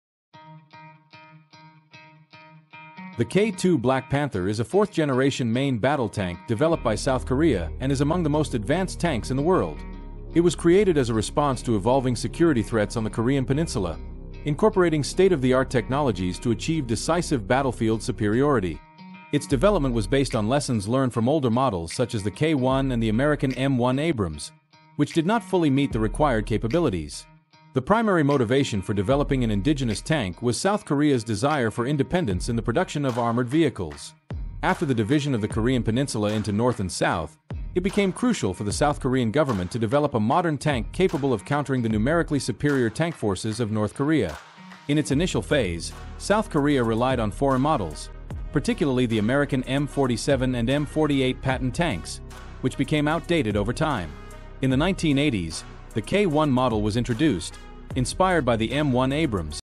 MBT K2 Black Panther. The best tank in the world? Military equipment
K2 Black Panther is the main fourth-generation combat tank developed by South Korea and is among the most advanced tanks in the world. It was created as a response to the changing security threats on the Korean Peninsula, and its construction incorporated the latest technologies to gain a decisive advantage on the battlefield. The development also utilized insights that older models, such as the K1 or the American M1 Abrams, could not adequately cover.